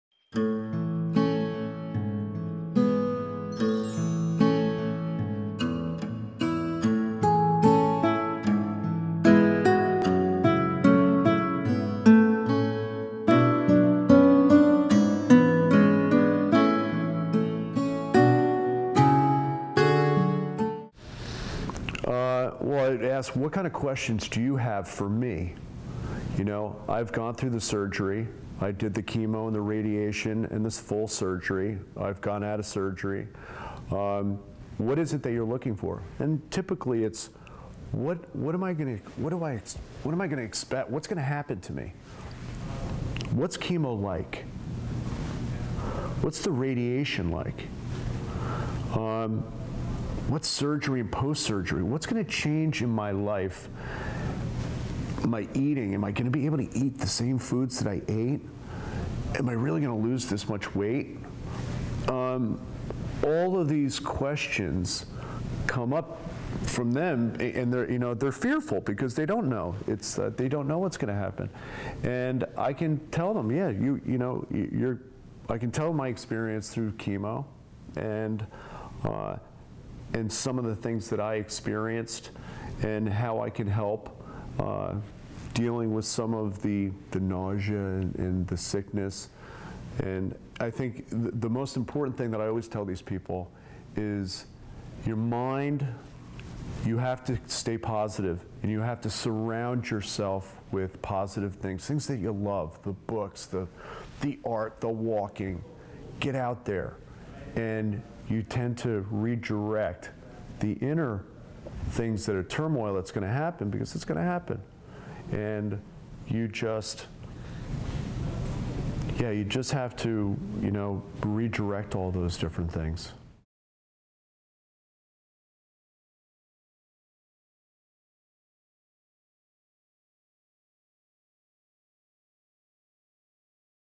Mark Speckhart | Part 12 | My Role as a Cancer Advocate
Mark talks about how he tries to be a good advocate for the patients he helps. He asks them about the questions they might have for him, since he has already experienced chemotherapy, radiation, surgery, and is maintaining a positive life after surgery.